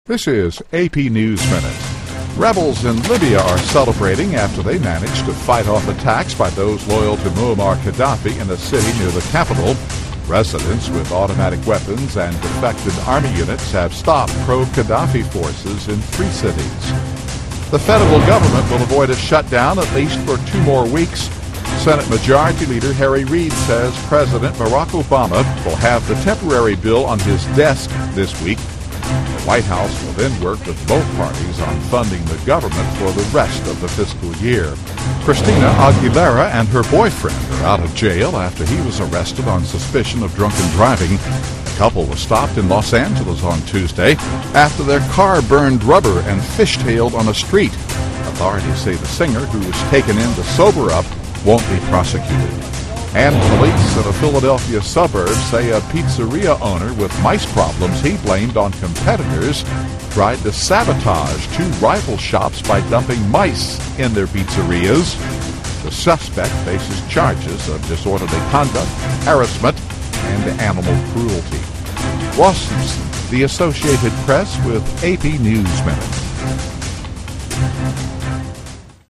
AP Top Stories
Here's the latest news for Tuesday, March 1:  Pro-Gadhafi forces turned back in city near the capital; House passes bill to avert shutdown; Christina Aguilera, boyfriend arrested; Pizzeria owner accused of planting mice at rival shops.